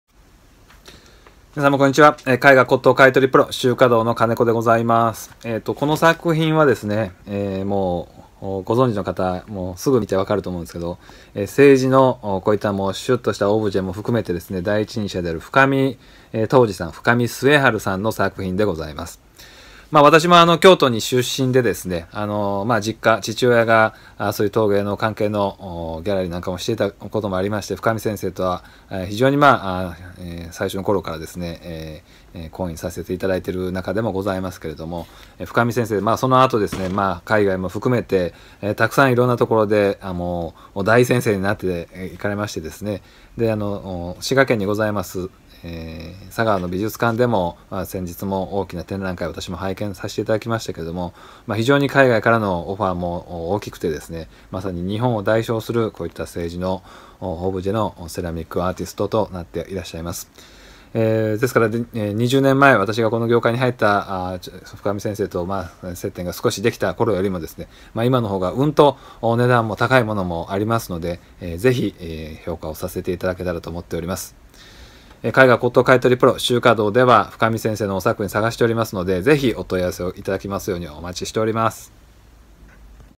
現代陶芸 深見陶治の青白磁 壺やオブジェを高く買取しています【絵画骨董買取プロby秋華洞】陶器もお任せください！
京都の伏見にお住いの深見先生には、私の父も大変お世話になっている陶芸家です。佐川美術館の展覧会も素敵でした。中東諸国をはじめ、深見コレクションは世界に広がっています！